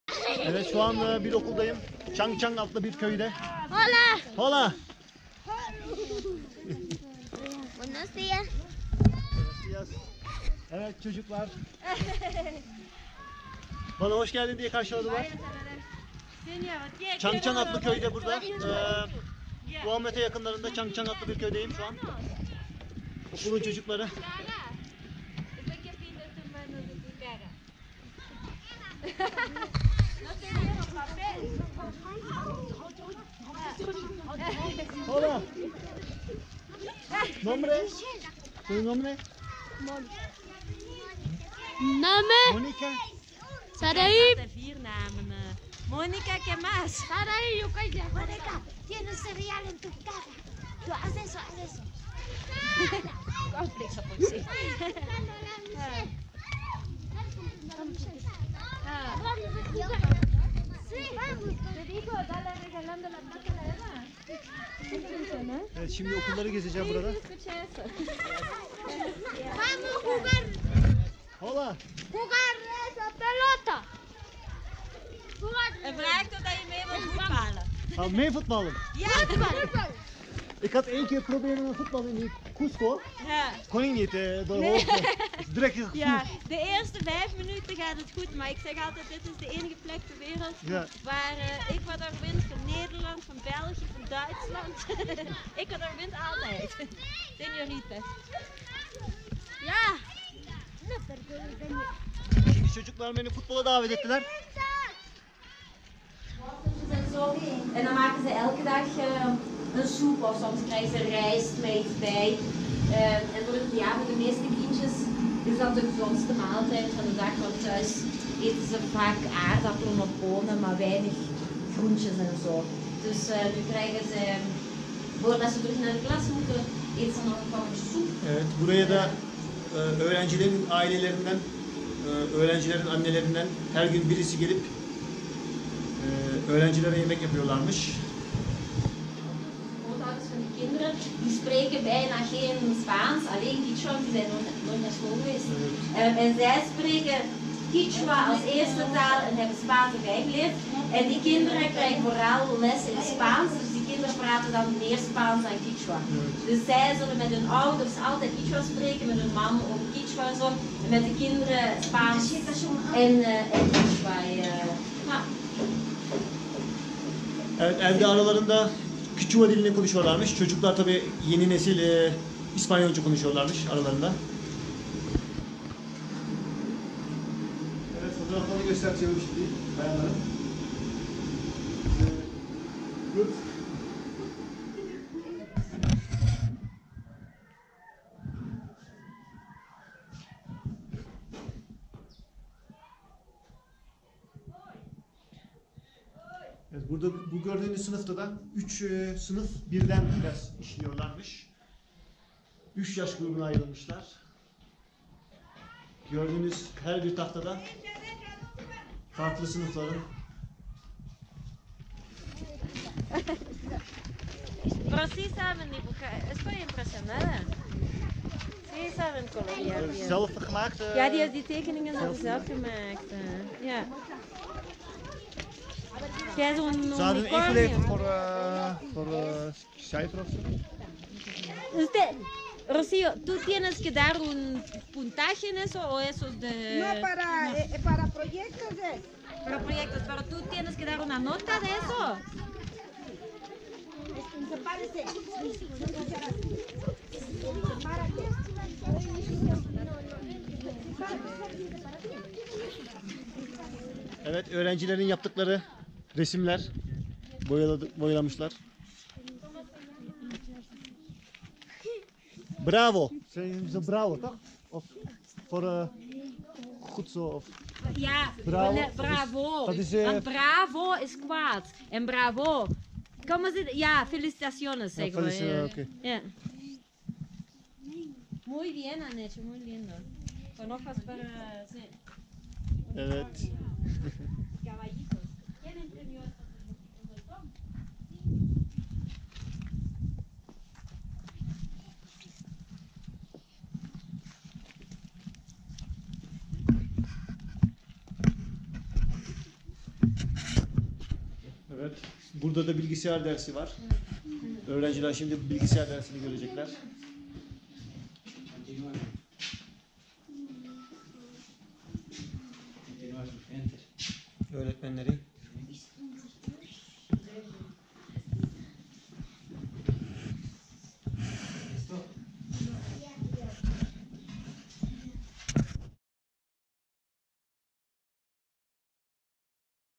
Ekvador Guamote yakınlarında ki Chan Chan adlı köyün okuluna yapmış olduğum ziyaretim
(TR) Ekvador Guamote yakınlarında ki 4000 metre rakımlı Chan Chan adlı köyün okuluna ziyarete gittiğimde, okulun öğrencileri bana yakın ilgi göstermişlerdi.
Bir sınıfın 3 ayrı yazı tahtasıyla üçe bölünüp, okulun tüm öğrencilerinin aynı sınıfta nasıl zor şartlarda eğitim aldıklarını ve bu okulda ki öğrencilerin annelerinin imece usulü sırasıyla öğrencilere yemek yaptığı okul mutfağınıda zor şartlarda nasıl gönüllü olarak çalıştıklarını yerinde gözlemleyip fotoğraflamıştım.

(NL) Ik werd open armen ontvangen door de kinderen tijdens mijn school bezoek bij het dorp Chan Chan in de buurt van Guamote in Ecuador.
Hier op school heb ik gezien hoe de kinderen in slechte omstandigheden onderwijs moesten volgen. Alle kinderen waren verdeeld in 3 klassen door zijn leeftijden, maar ze allen krijgden tegelijkertijd les in één zelfde klas die verdeeld waren door 3 schrijfborden. Ik heb hier ook de school keuken geweest waar door de moeders van kinderen om de beurt vrijwillig gekookt werden.